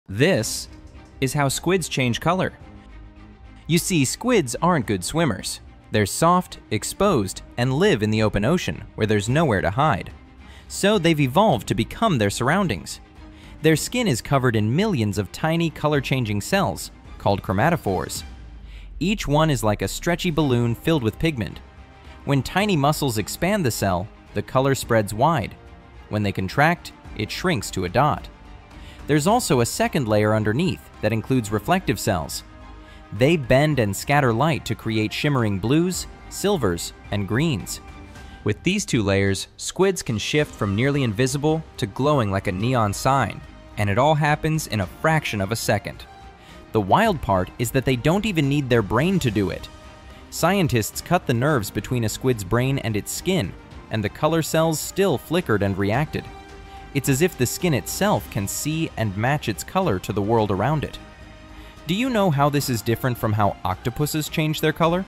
How Squids Instantly Change Color 🦑✨
This — is how squids change color! 🦑✨
In the open ocean, they can’t hide behind rocks or coral, so they’ve evolved to become their surroundings. Their skin is covered in millions of color-changing cells called chromatophores, each one a tiny balloon of pigment that stretches or shrinks in an instant. Beneath them lies another layer of reflective cells that bend light into shimmering blues, greens, and silvers. Together, they let squids vanish, flash, or glow like neon signs — all in milliseconds. Even stranger? Their skin keeps changing colors without their brain. It’s like the skin itself can see.

🎁 Explore my hand-picked gear, books, and gifts for animal lovers →

Discover fascinating animal facts and science in fun videos.